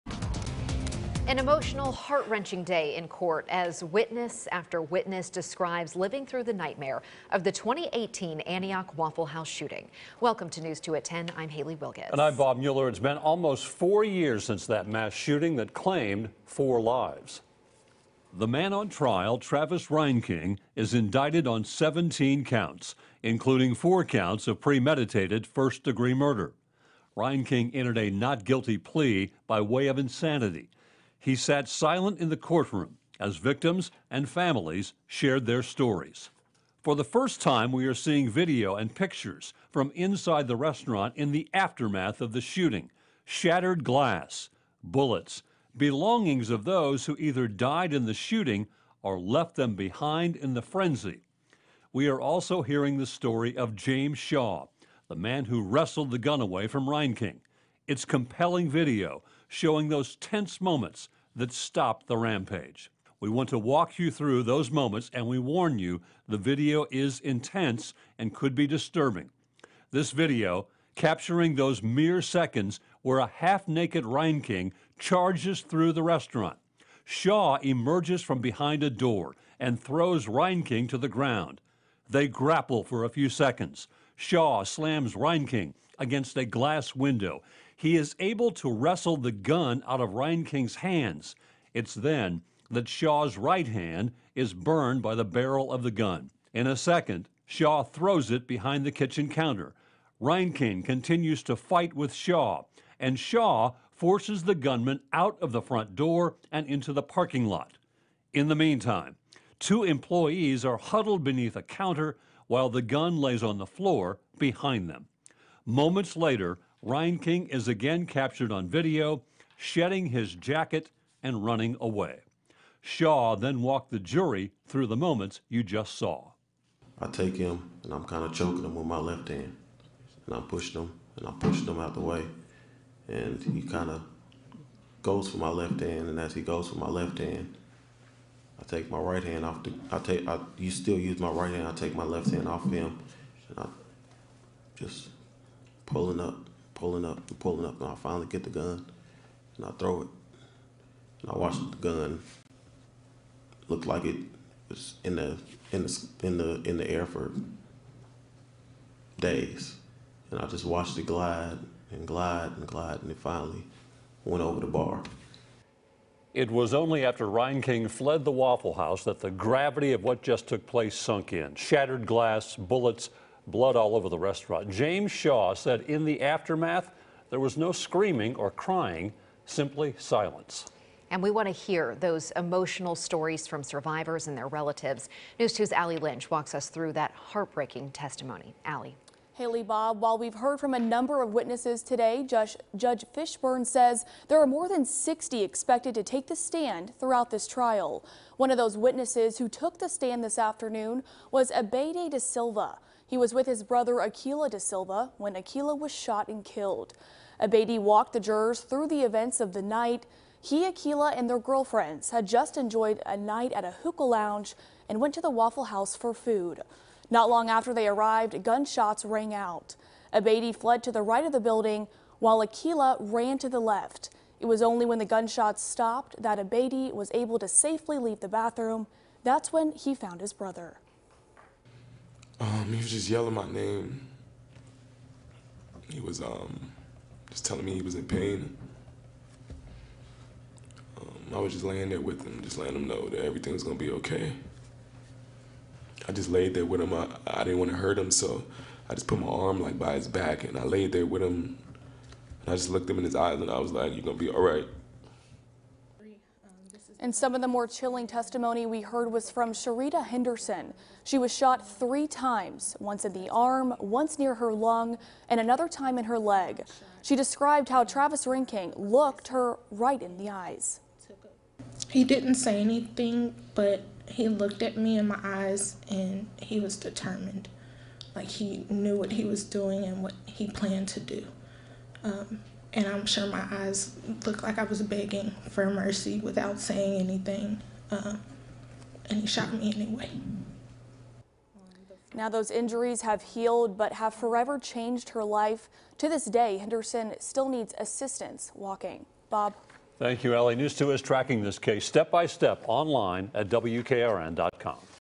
Surveillance video of ‘Waffle House Hero’ shows intense moments he wrestled rifle away from suspect
Gruesome, horrifying and emotional moments were revealed on day one of the 2018 Antioch Waffle House shooting trial.

• Find today’s top stories for Nashville, TN and all of Middle Tennessee